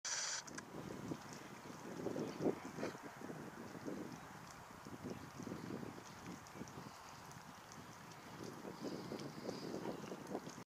Baby Egyptian Geese
A short clip ofsome baby Egyptian  geese at Attenborough  nature  reserve  Nottingham